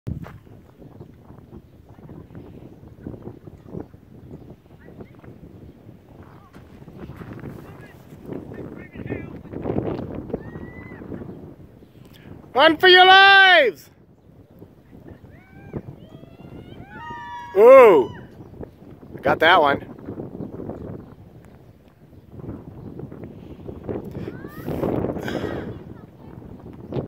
Spring storm ride
Heading back from ride. Storm looming in